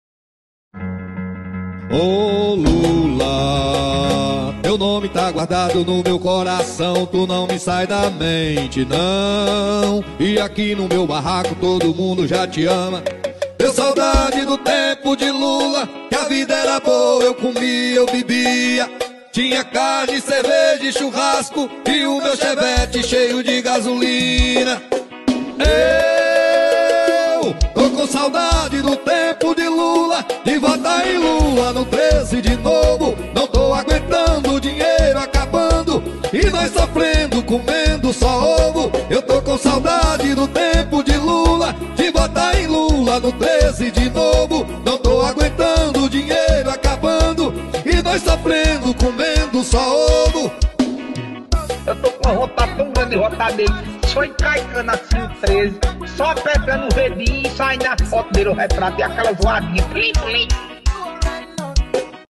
Uma projeção em Fortaleza que lavou a alma do Nordeste
Uma projeção em Fortaleza (CE) lavou a alma do Nordeste, após a onda de ofensas estimulada pelo miliciano do Planalto. Assista. Quem tem memória e conhece a história sabe o que é melhor para o seu país!